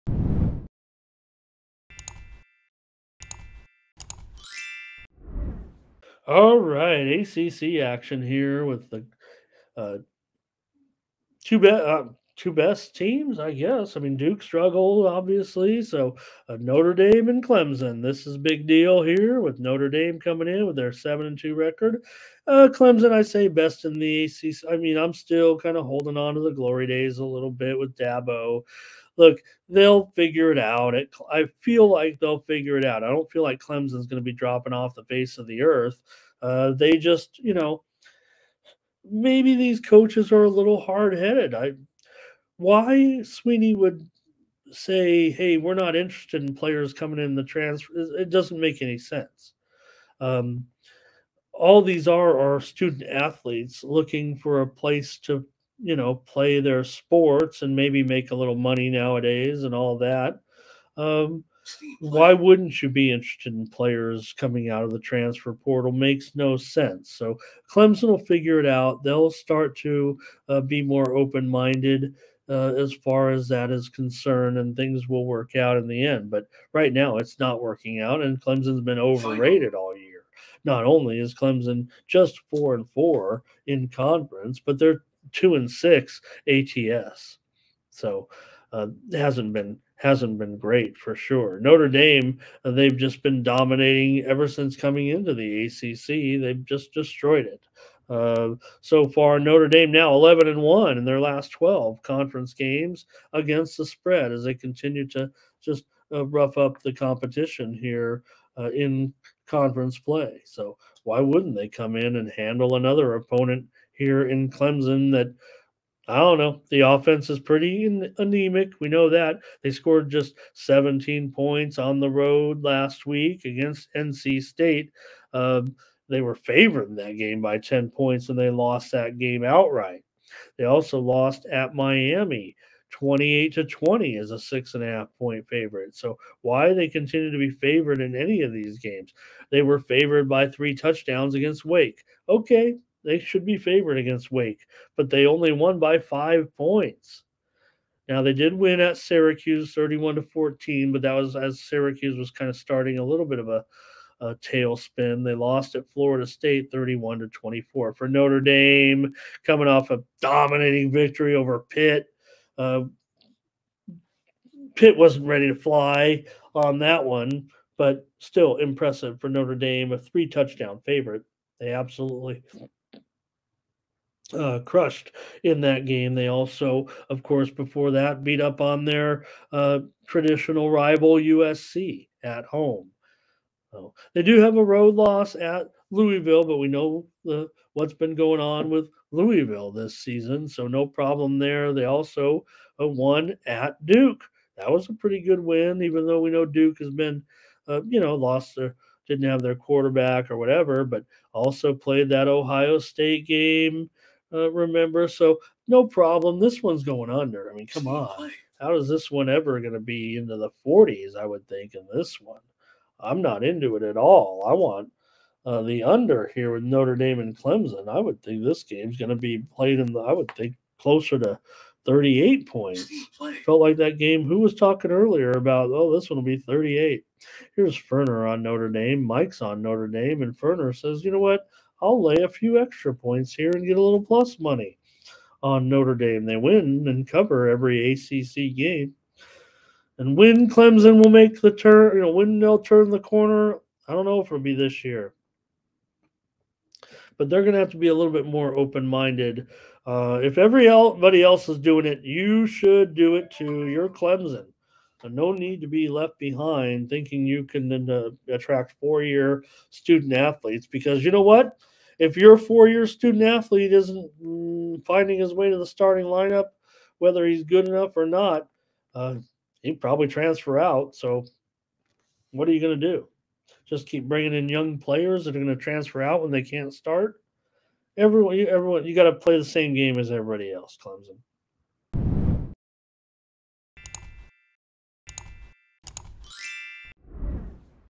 Notre Dame vs Clemson 11/4/2023 Week 10 FREE College Football Picks and Prediction on Morning Steam
11/4/2023 Notre Dame vs Clemson FREE College Football Picks and Prediction on Morning Steam Show